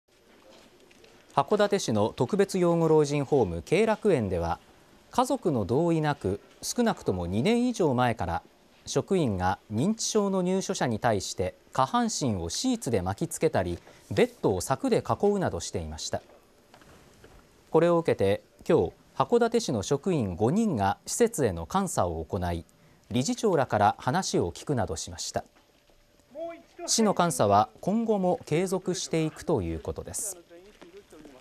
函館市が特養老人ホームを監査 家族の同意なしに入所者の身体を拘束していた問題で 今後も監査を継続
函館市の特別養護老人ホームで家族の同意なしに入所者の身体を拘束していた問題を受け、１５日に市は施設への監査を行いました。

函館市の特別養護老人ホーム恵楽園では、家族の同意なく少なくとも２年以上前から、職員が認知症の入所者に対して、下半身をシーツで巻きつけたり、ベッドを柵で囲うなどしていました。これを受けて１５日に函館市の職員５人が施設への監査を行い、理事長らから話を聞くなどしました。市の監査は今後も継続していくということです。